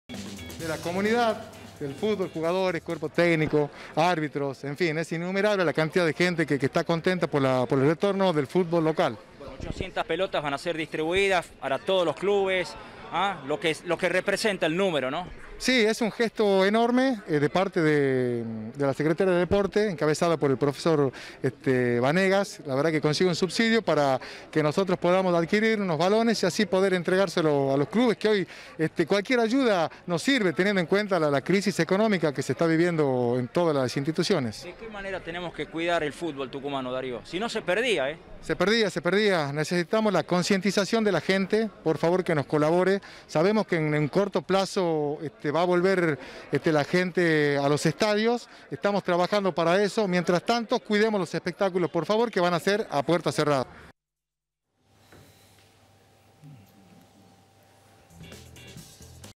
ARRANCO LA LIGA TUCUMANA DE FUTBOL
EN EL PARTIDO INAUGURAL CENTRAL NORTE VENCIO A SAN MARTIN 2 A 1